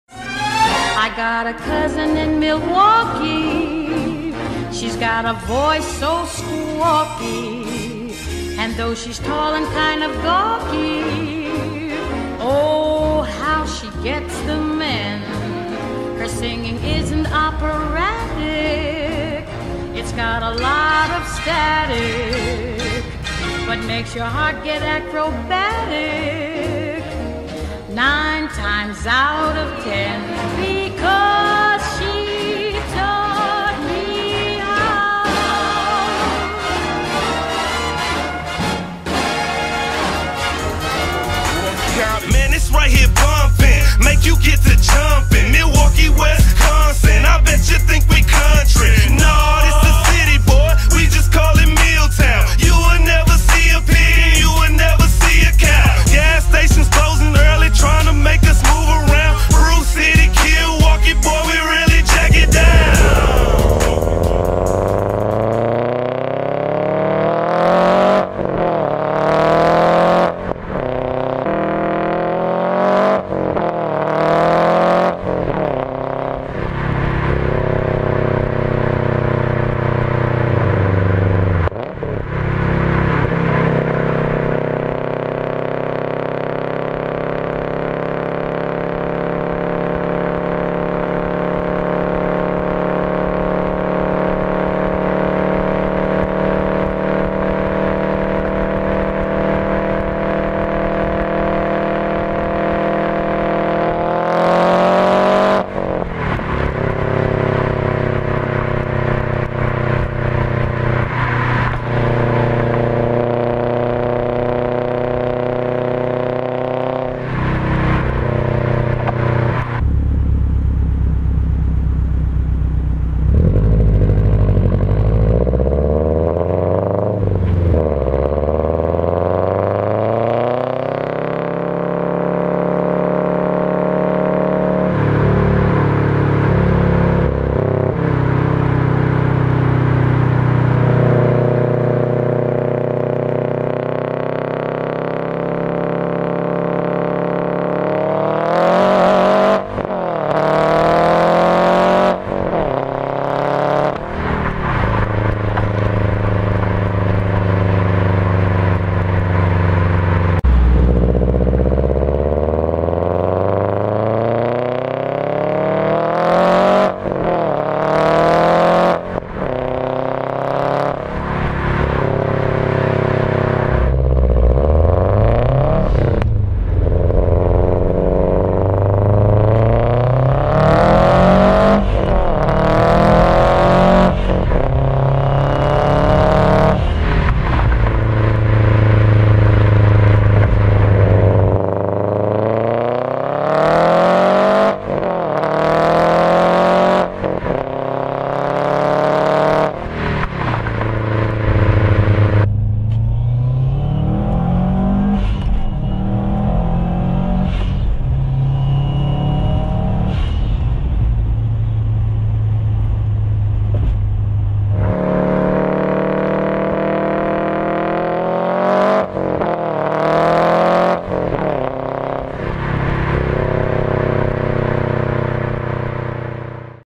Midnight In Milwaukee | Subaru Forester XT with Blouch 18G-XT | Pure Sound
Some pulls in Milwaukee at night.

~Vehicle Info~
2004 Subaru Forester XT
5 speed manual transmission
Built Motor
Blouch 18G-XT
2" Lift
and a bunch more